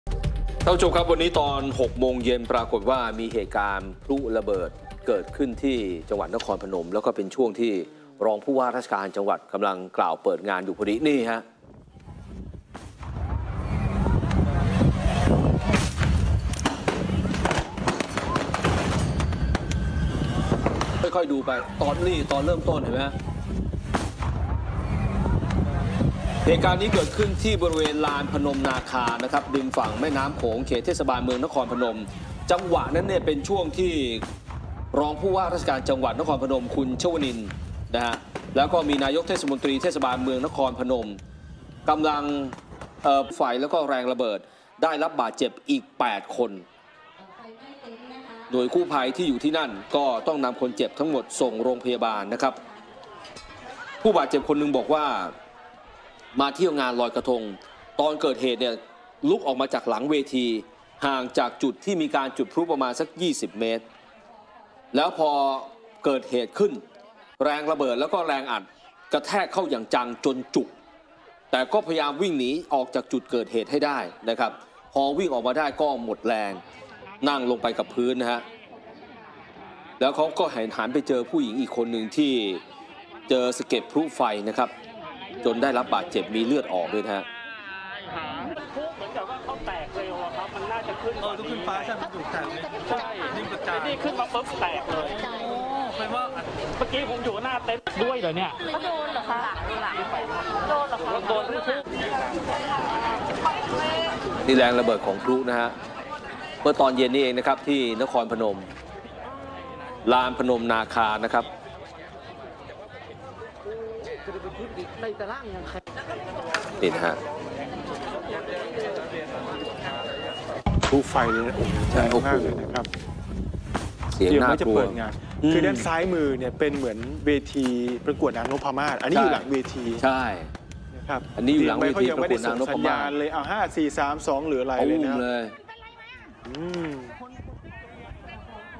เปิดคลิปสยอง!พลุไฟบึ้ม เจ็บเพียบ | เล่าข่าวข้น | TOP NEWS
สยอง! เปิดคลิปพลุไฟบึ้มกลางงานลอยกระทง คนเจ็บนับสิบ